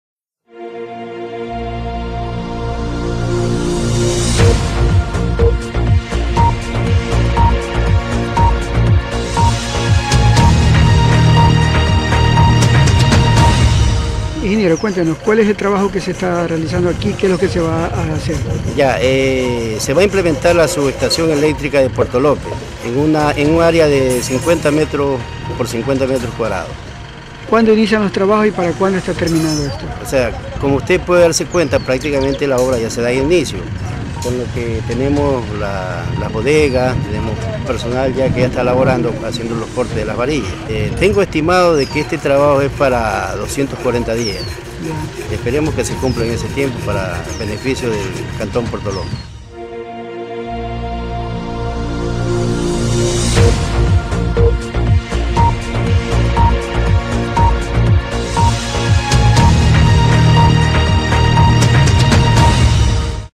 LUIS CEDEÑO - ENCARGADO DE OBRA PARA SUBESTACIÓN DE ENERGÍA ELÉCTRICA
INICIO DE OBRA  MEJORAMIENTO SUBESTACIÓN  DE ENERGÍA ELÉCTRICA PUERTO  LOPEZ